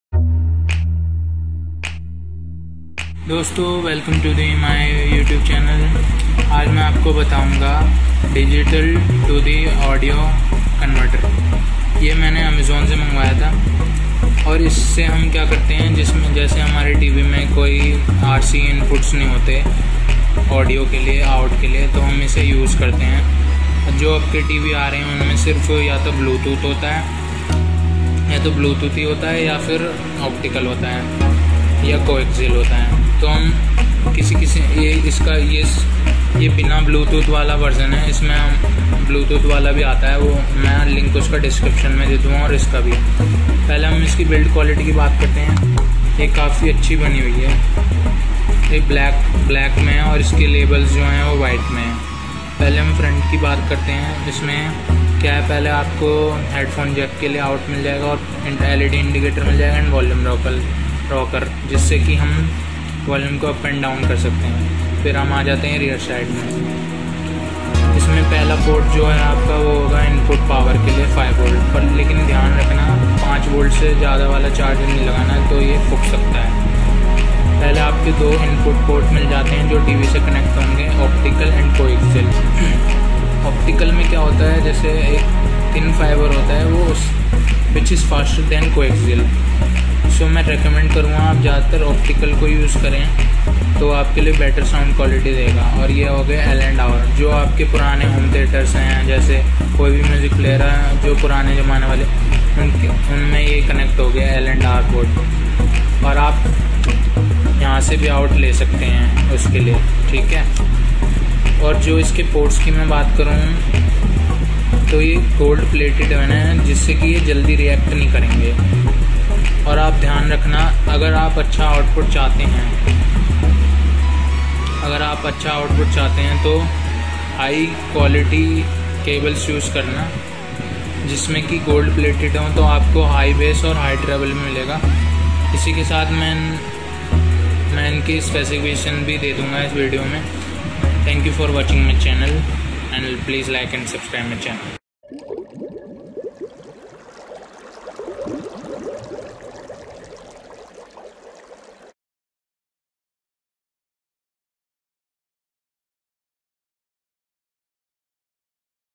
digital to analog audio converter
hello guys this video based upon electronics device
digital to analog video converter
this convert digital signal to analog signal

Buying links are given below:-

1.Tobo 5.1CH Digital Audio Converter 192 KHz DAC Converter HD HiFi Optical to RCA + 3.5mm Headphone Jack Converter.

2.192Khz BT DAC Digital to Analog Audio Converter with BT Receiver with Volume Control

and be safe during this pandemic.